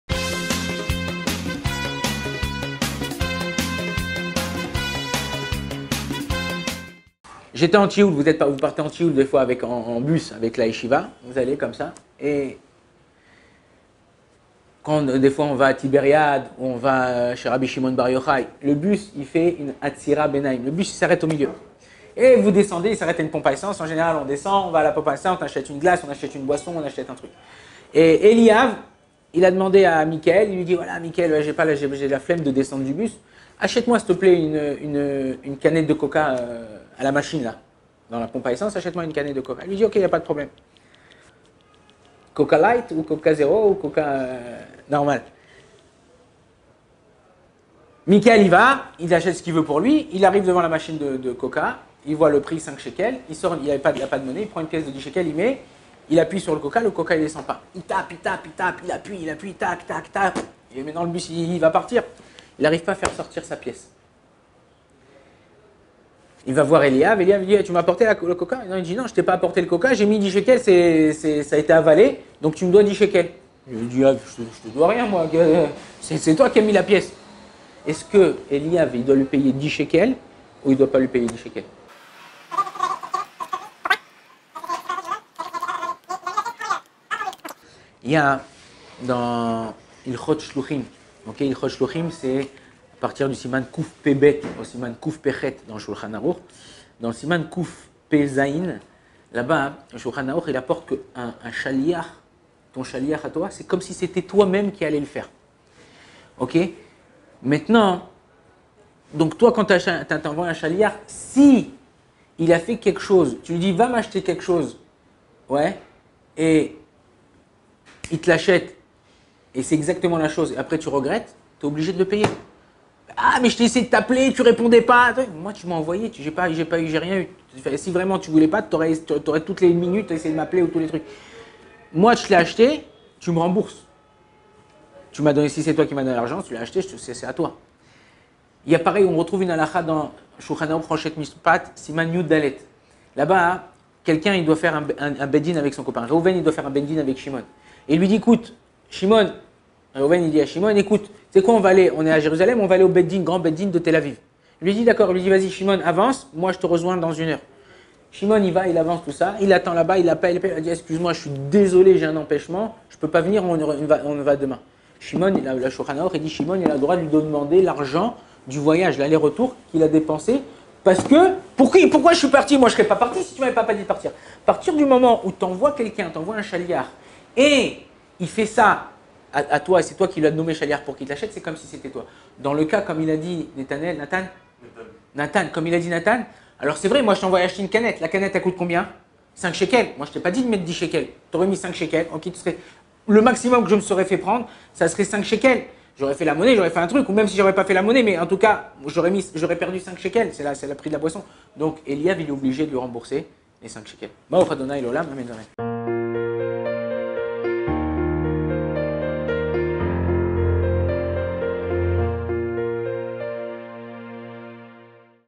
Achète-moi un Coca - Rav Ankry
Tu m'as demandé de t'acheter un Coca, j'ai mis 10 shekels dans la machine et ... pas de coca et plus d'argent.
J'ai perdu mes 10 shekels !
Oui ... mais moi j'ai pas reçu mon coca !
Que faire ?
Une étude sur la loi juive de l'émissaire et ses conséquences financières.
A combien de cas comparables sommes-nous confrontés, sans savoir que dit la loi juive à ce sujet ?

♻ à partager au plus grand nombre... ♻

✅ Posez une question confidentielle par Whatsapp au Rav Ron Chaya.
Il vous répond par note vocale. Pensez à préciser votre prénom et votre nom